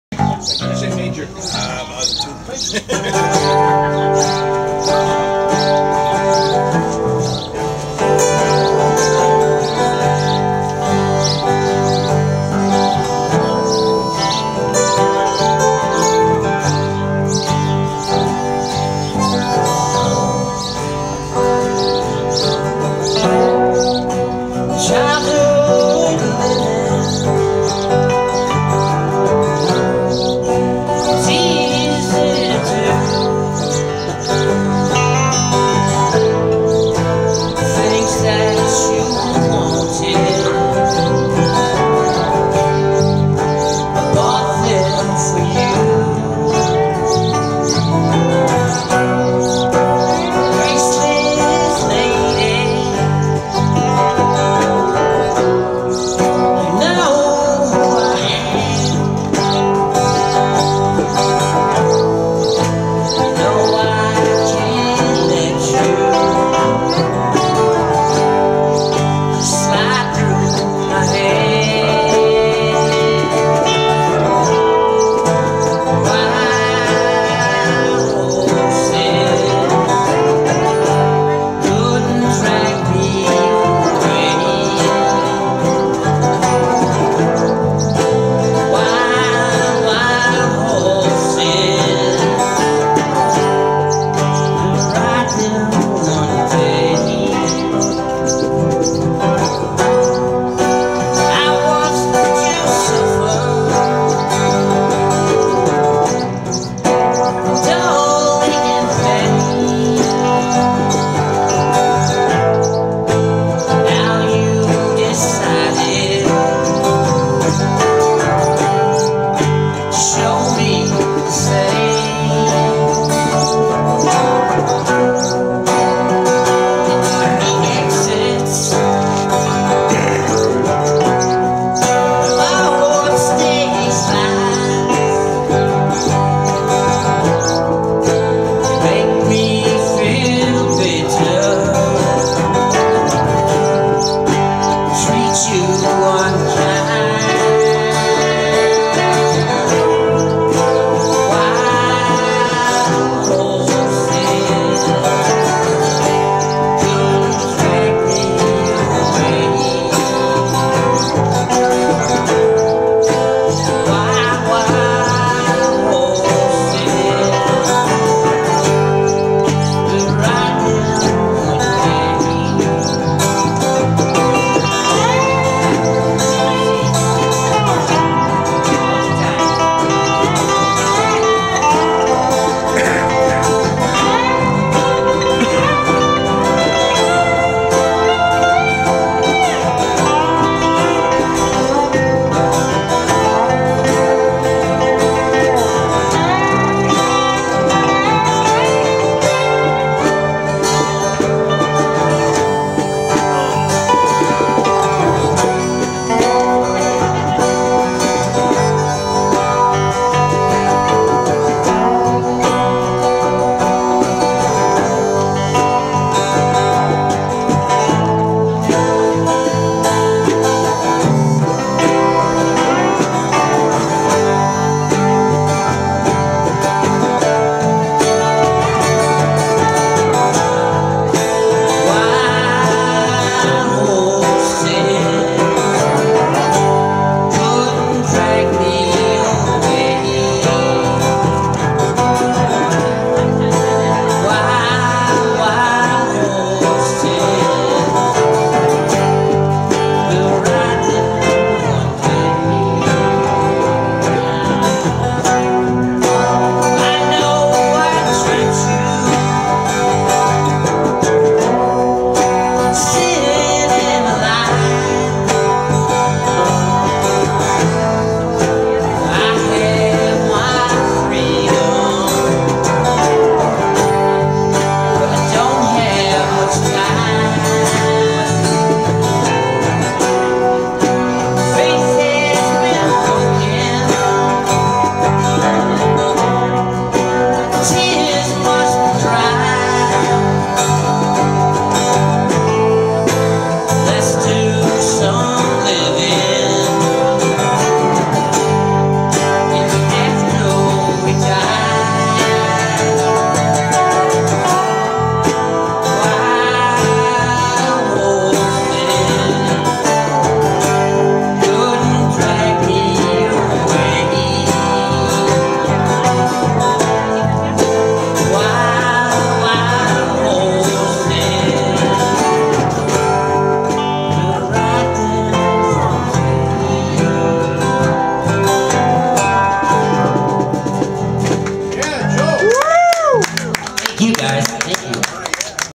Newburgh Jammers - Joel and Bob patterson
Memorial day 2018 jam.